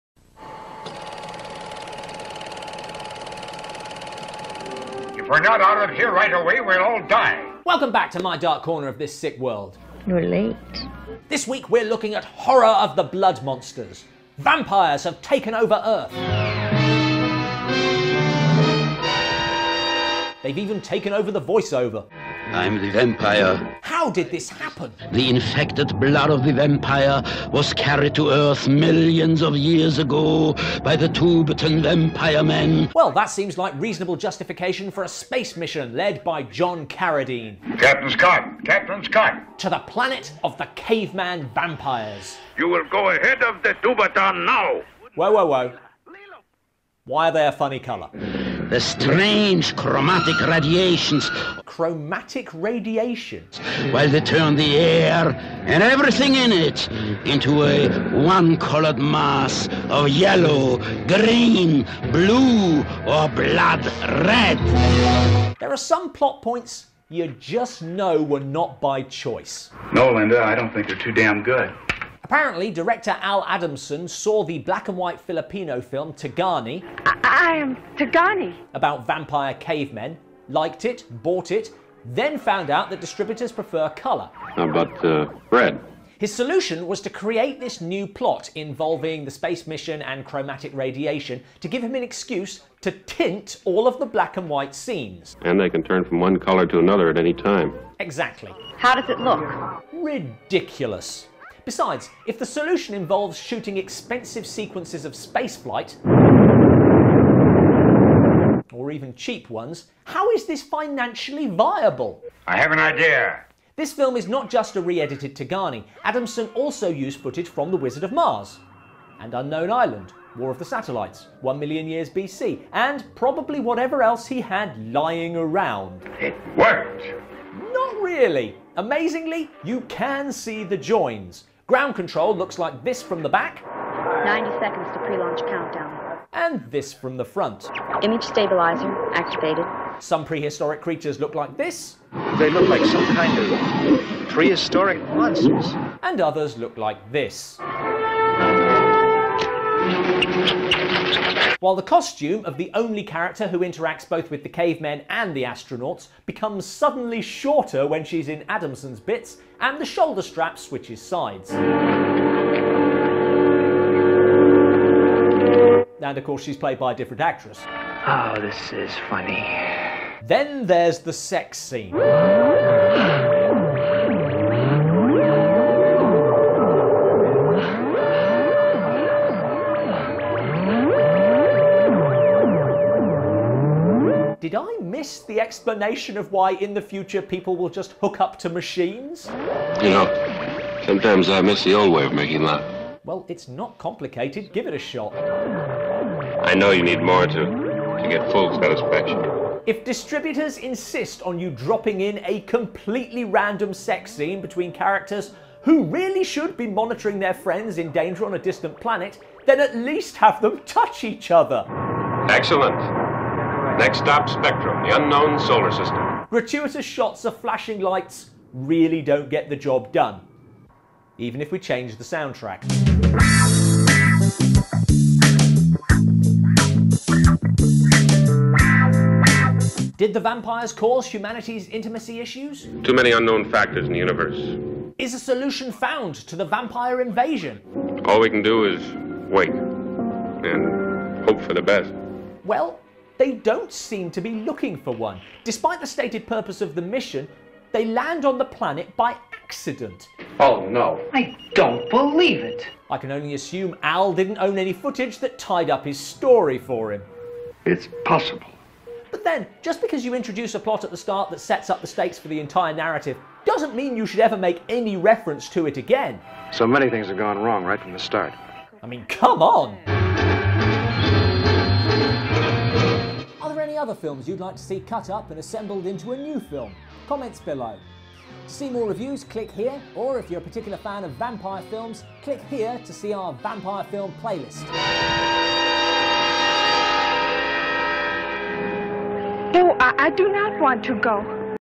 Dark Corners - Horror of the Blood Monsters: Review
You'll scream yourself into a state of shock! We review Horror of the Blood Monsters.

Summary: In the near future with a intergalactic vampire plague threatening earth, an expedition is sent to a distant galaxy in hopes of discovering the plague^Òs source. Landing on a mysterious planet they discover that Spectrum radiation has turned the atmosphere into a one-color tint. Exploring further, the group discovers living dinosaurs, a race of vampire cavemen, and other strange creatures.

Written by & starring Robin Bailes @robinbailes
Directed and Edited by Graham Trelfer @thelcw
DARK CORNERS OF THIS SICK WORLD S05E